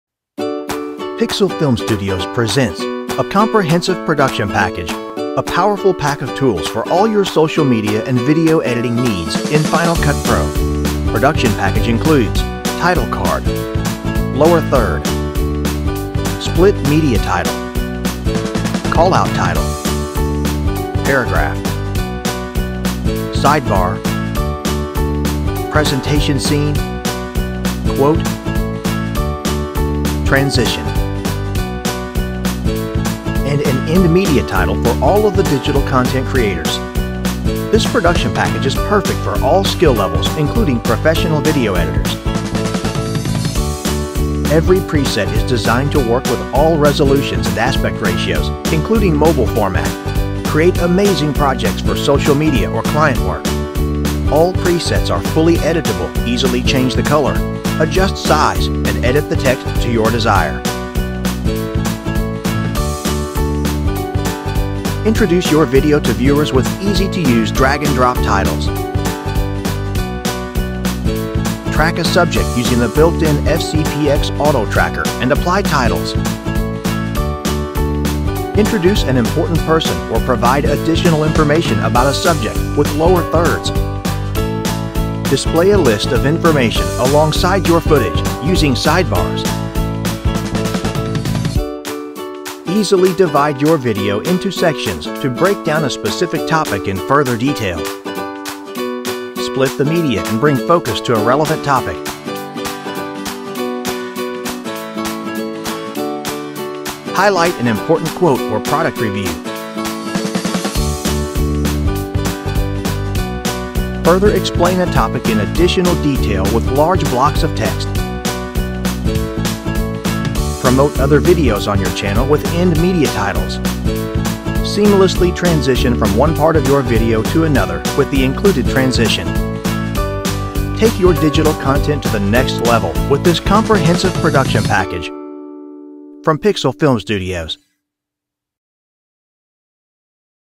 Geometric Display - Comprehensive Pack of Tools for FCPX - Pixel Film Studios
Geometric Display is a powerful pack of tools for all your social media & video editing needs. These can all be found directly inside of Final Cut Pro. This package includes 10 Presets that are designed to work in conjunction with each other. As a result this will bring continuity to any video production.

Whether you are just getting started as a video editor or you are a seasoned professional this pack has everything you will need to take your next project to an even higher level.

A Powerful Pack of Tools

Geometric Display Production Package is an excellent package of tools for all your social media and video editing needs. They are all found directly inside of Final Cut Pro. This package includes 10 Presets that are designed to work with one another to bring continuity to any video production.

Comprehensive Production Package

Geometric Display includes everything you will need for all of your video projects. It comes packed with a Title Card, Lower Third, Split Media Title, Call-Out Title, Paragraph, Sidebar, Presentation Scene, Quote, Transition, and End Media Title!

The Geometric Display Production Package is designed for digital content creators of all skill levels from beginners to professionals. If you’re a YouTuber, Freelancer, or Professional content creator this package will save you time and money.

Any Resolution/Media Types

Geometric Display is compatible with all resolutions and aspect ratios including HD, 4K, 5K, and mobile formats. These presets can be applied to any media making it extremely flexible across all your social media and editing platforms.